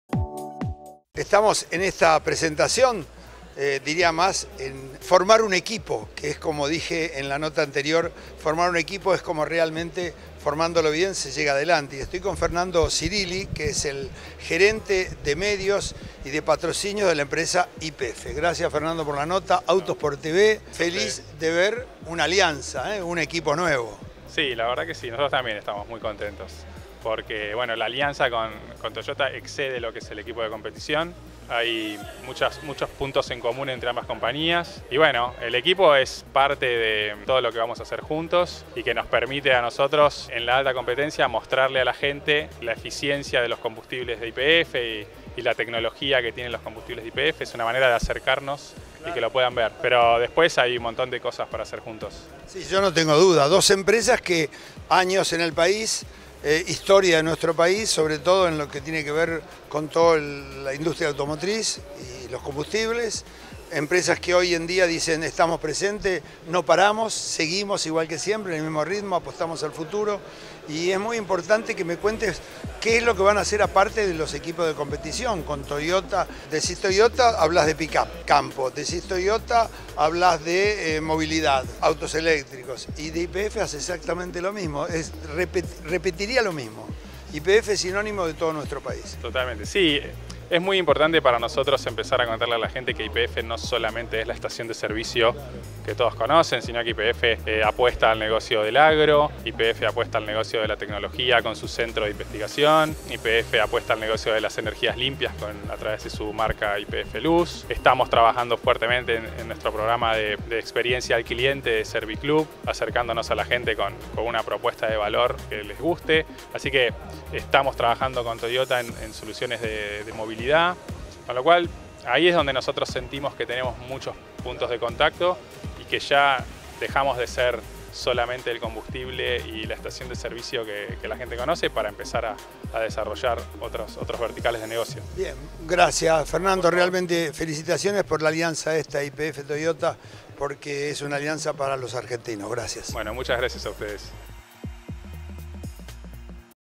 Fernando Cirilli "En la alta competencia podemos acercarnos a la gente"
Cocho Lopez, habló con Fernando Cirilli, Gte. de medios y patrocinios de YPF, habla sobre la alianza entre YPF y Toyota y mucho más.